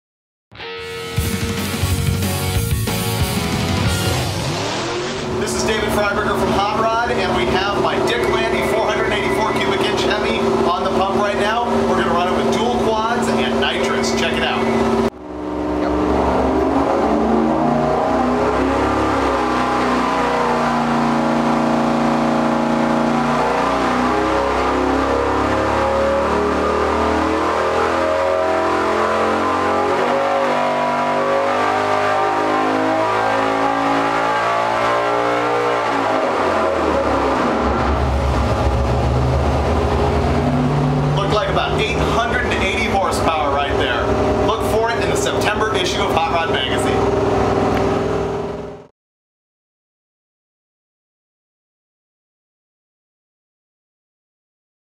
David Freiburger's Dick Landy 484ci Hemi Engine Dyno Run
Down at Westech we run Freiburger's Dick Landy's 484 Hemi with dual quads and nitrous to see how much horsepower we can get out of it. Check it out you'll be blown away from the numbers we pull.